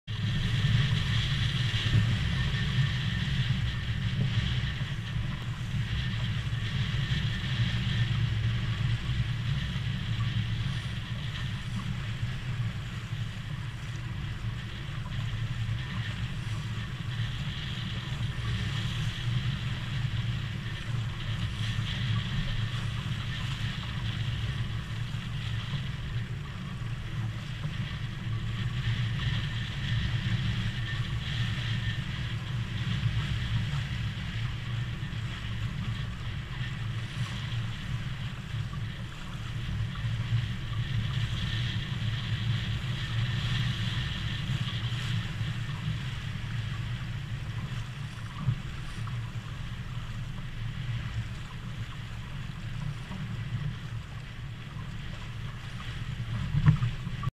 Contessa 28
Short impression of a solo sail with my previous Contessa 28
Winters solotochtje met mijn Contessa 28